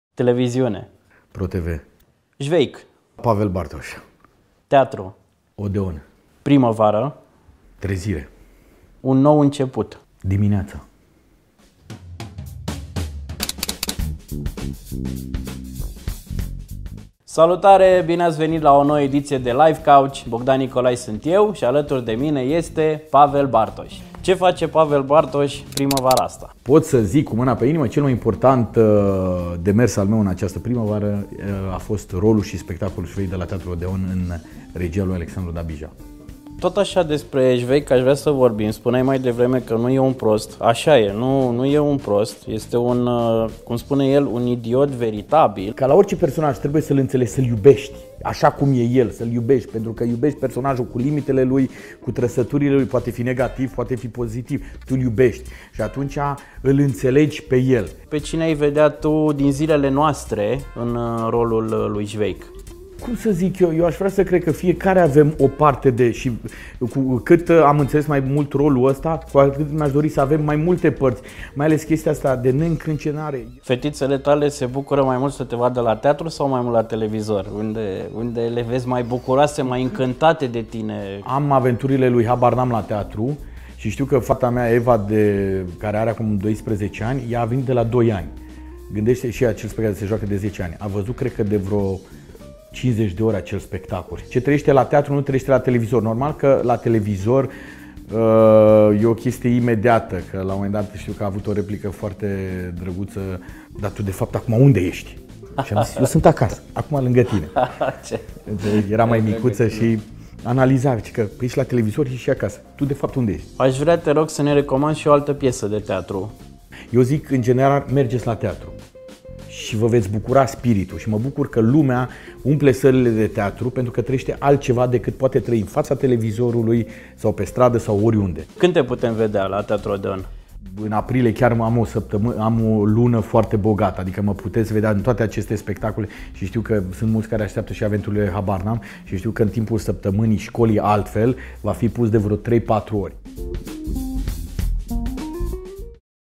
VIDEO – LIFE COUCH cu Pavel Bartoș
În luna martie, Pavel Bartoș este soldatul Švejk la Teatrul Odeon. Cât de greu a intrat în pielea personajului și cât de ușor îi este să joace marile roluri ale vieții sale: tată, soț, actor și om de televiziune.